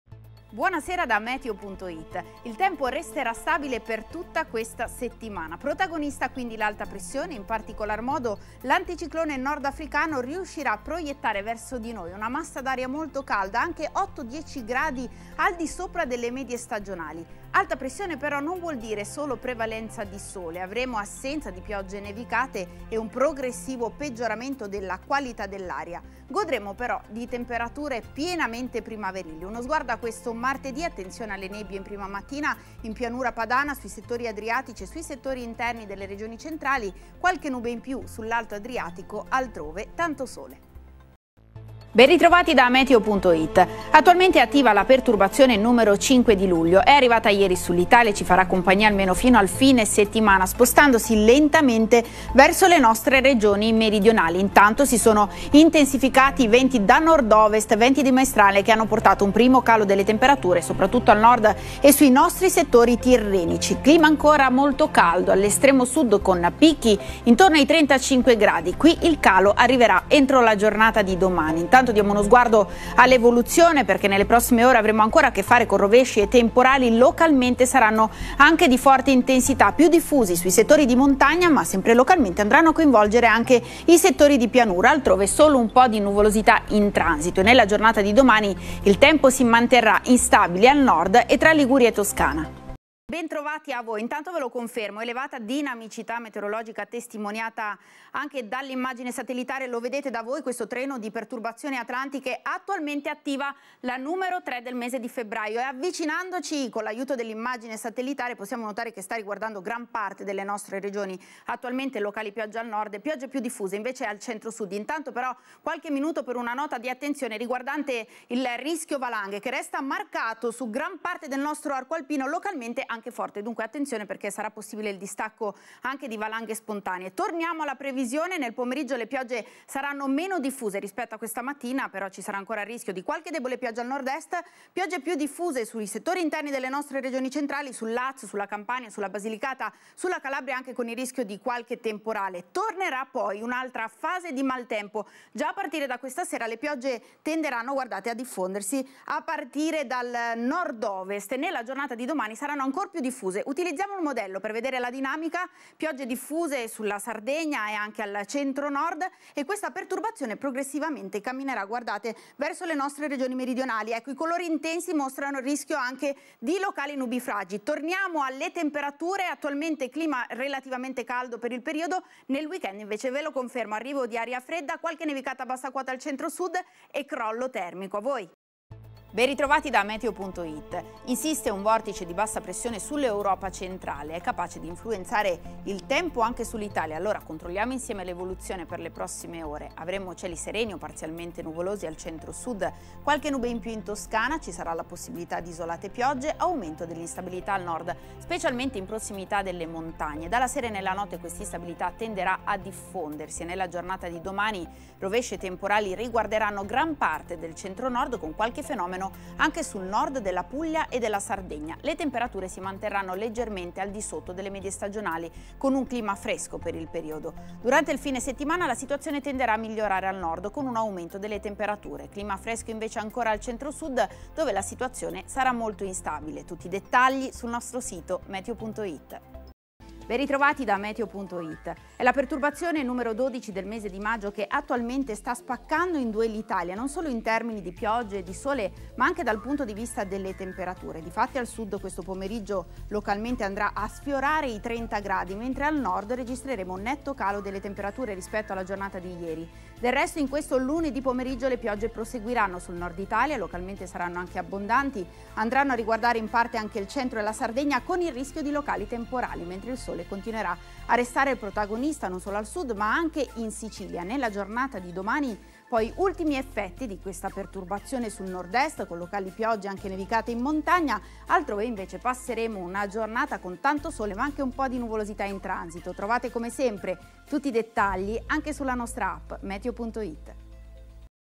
Serena Giacomin
Young but already established weather expert, she recently published an interesting book about climate change fake news: definitely not the usual cover girl chosen for her nice looking only, she's a skilled tv host knowing well her way around. Nevertheless, her distinctive charm is always enthralling: classy blazer and black pants or an elegant formal dress, she never goes unnoticed